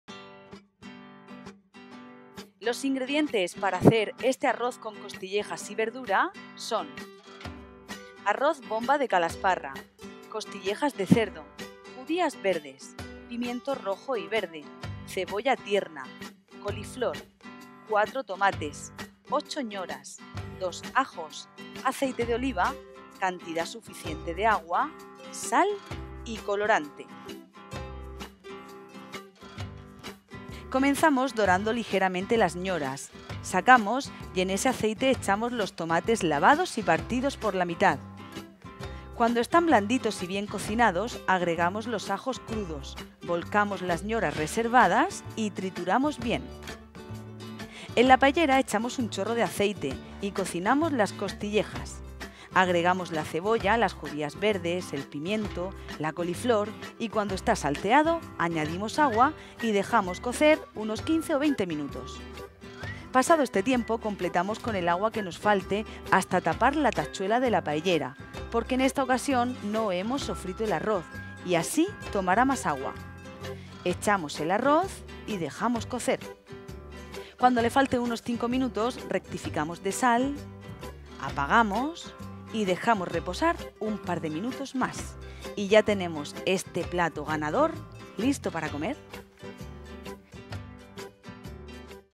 Arroz con costillejas y verdura
Carmela Santana Rodríguez

INGREDIENTES:
Arroz bomba de Calasparra
Costillejas de cerdo
Judías verdes
Pimiento rojo y verde
Cebolla tierna
Coliflor
4 Tomates
8 Ñoras
2 ajos
Aceite de oliva
C/s de agua
Sal
Y colorante

ELABORACIÓN:
Comenzamos dorando ligeramente las ñoras, sacamos y en ese aceite echamos los tomates lavados y partidos por la mitad. Cuando están blanditos y bien cocinados agregamos los ajos crudos , volcamos las ñoras y trituramos.
En la paellera echamos un chorro de aceite y cocinamos las costillejas, agregamos la cebolla y las judías verdes, el pimiento, la coliflor, y cuando esta salteado añadimos agua y Dejamos cocer unos 15-20 minutos. Pasado este tiempo completamos con el agua que nos falte hasta tapar la tachuela de la paellera porque en esta ocasión no hemos sofrito el arroz y tomará más agua.
Dejamos cocer y cuando le falten unos 5 minutos rectificamos de sal. Apagamos y Dejamos reposar un par de minutos.

Y en Twitter: @ensupuntoconbar